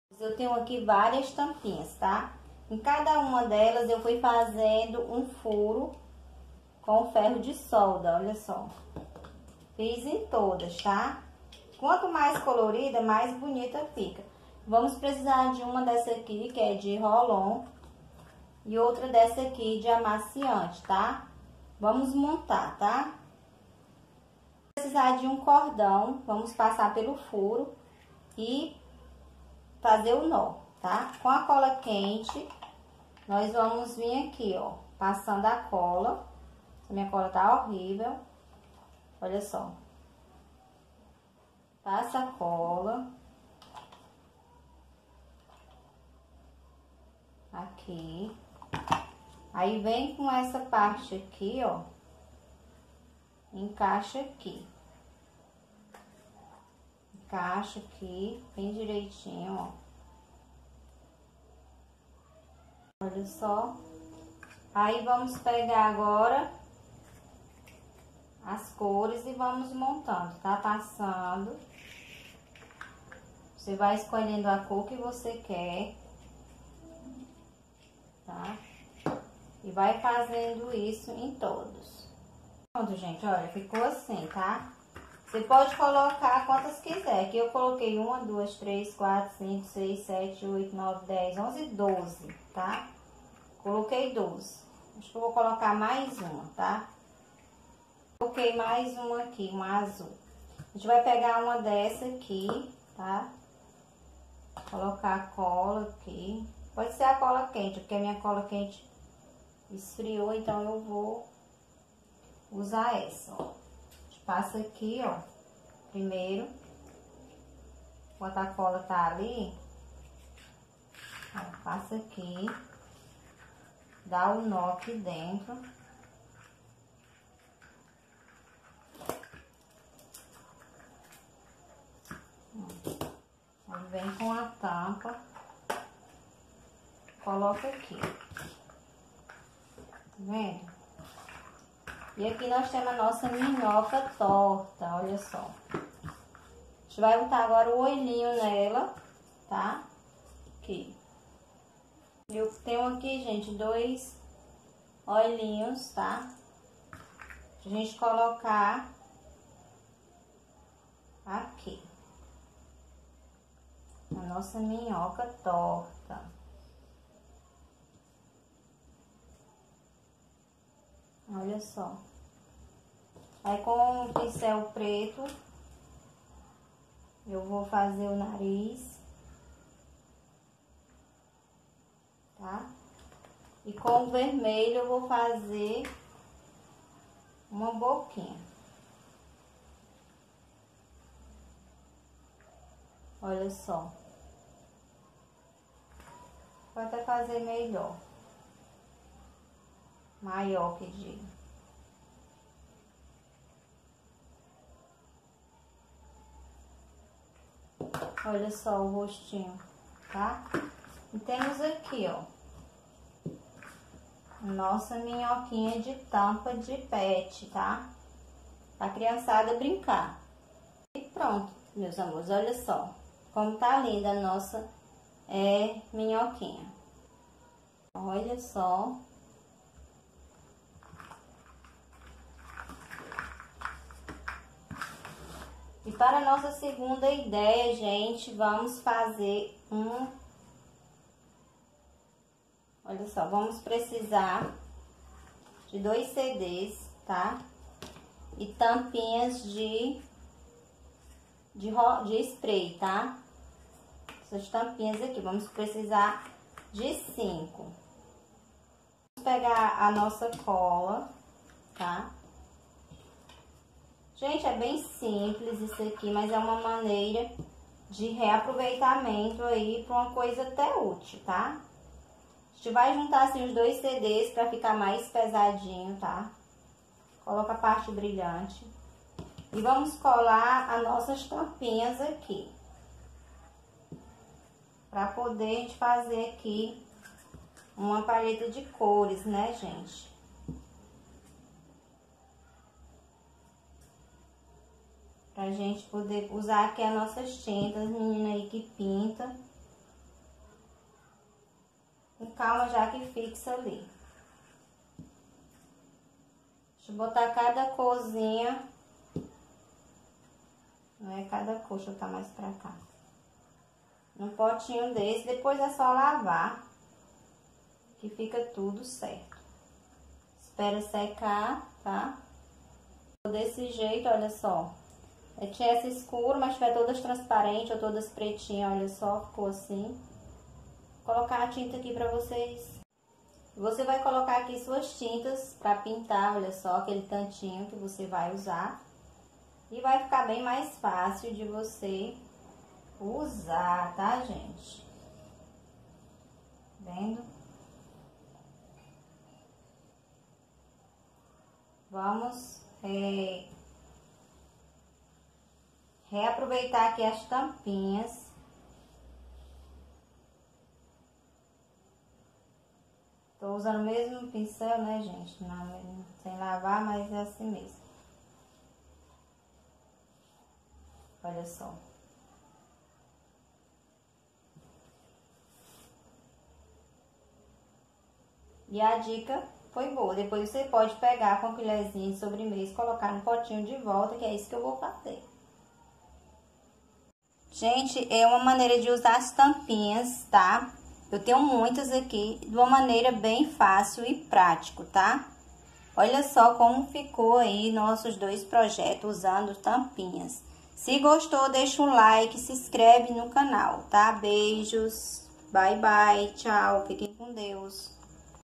2 ideas com tampas
Olá amores, o vídeo de hoje é mais duas ideas com tampas. Gostou do vídeo?

QUER MANDAR CARTINHAS E PRESENTINHOS:

Edinete Araújo
Rua planaltina N° 1751 – Planalto Ayrton Senna
Cep: 60760480
Fortaleza-Ce